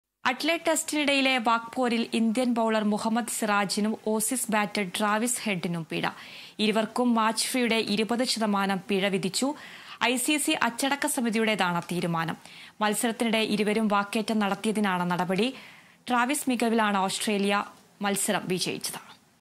അഡ്ലെയ്ഡ് ടെസ്റ്റിനിടയിലെ വാക്‌പോര്‌; മുഹമ്മദ് സിറാജിനും ട്രാവിസ് ഹെഡിനും പിഴ
അഡ്ലെയ്ഡ് ടെസ്റ്റിനിടയിലെ വാക്‌പോരിന് ഇന്ത്യന്‍ ബൗളര്‍ മുഹമ്മദ് സിറാജിനും ഓസീസ് ബാറ്റര്‍  ട്രാവിസ് ഹെഡിനും പിഴ. ഇരുവര്‍ക്കും മാച്ച് ഫീയുടെ 20 ശതമാനം പിഴ വിധിച്ചു. ഐസിസി അച്ചടക്ക സമിതിയുടേതാണ് തീരുമാനം. മത്സരത്തിനിടെ ഇരുവരും വാക്കേറ്റം നടത്തിയതിനാണ് നടപടി. ട്രാവിസ് മികവിലാണ് ഓസ്‌ട്രേലിയ മത്സരം വിജയിച്ചത്.

About COA